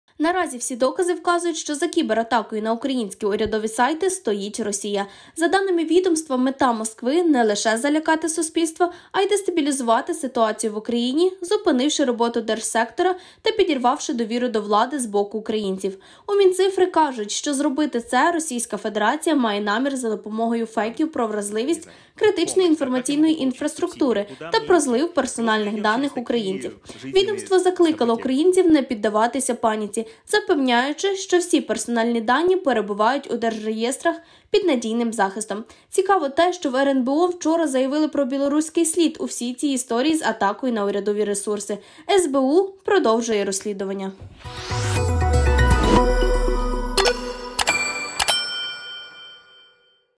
У Мінцифрі заявили: за кібератакою на Україну стоїть РФ
Наразі всі докази вказують, що за кібератакою на українські урядові сайти стоїть Росія.

За даними відомства, мета Москви — «не лише залякати суспільство, а й дестабілізувати ситуацію в Україні, зупинивши роботу держсектора та підірвавши довіру до влади з боку українців». У Мінцифри кажуть, що зробити це РФ має намір за допомогою фейків про вразливість критичної інформаційної інфраструктури та про "злив" персональних даних українців.

Відомство закликало українців не піддаватися паніці, запевняючи, що всі персональні дані перебувають у держреєстрах під надійним захистом.

Цікаво те, що в РНБО вчора заявляли про «білоруський слід» у всій цій історії з атакою на урядові ресурси.
СБУ продовжує розслідування.